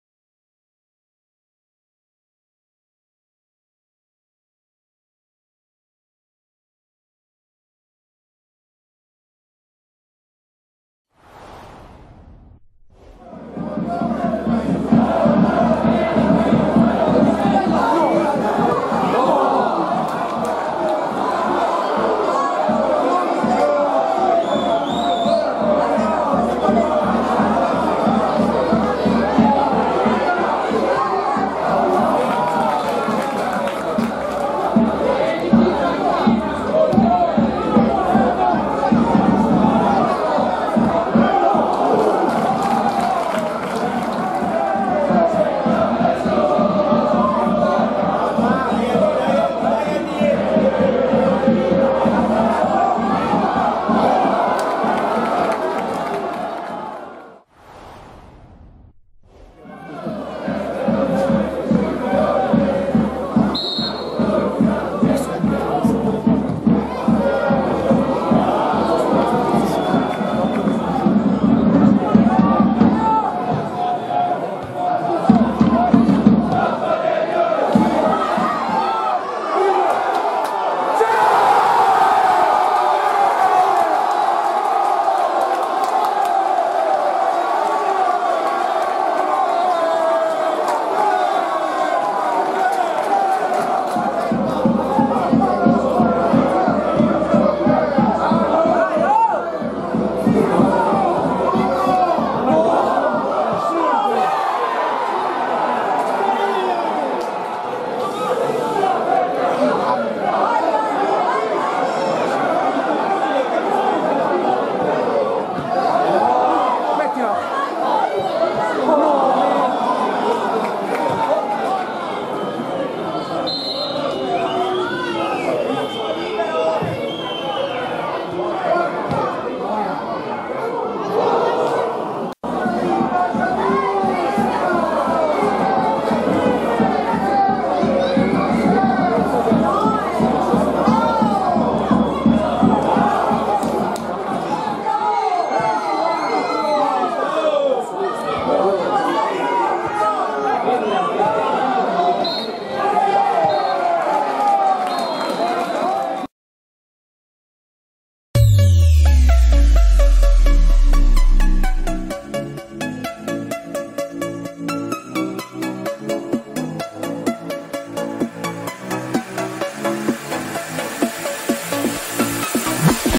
HIGHLIGHTS | Sant'Angelo - Ravenna FC 1-1
SANT'ANGELO - RAVENNA FC 1-1

Marcatori: 70’ Mecca, 80’ Pavesi

Credits by @ASDSantAngelo1907

Abbonati a questo canale per accedere e seguire in diretta le partite casalinghe del Ravenna FC: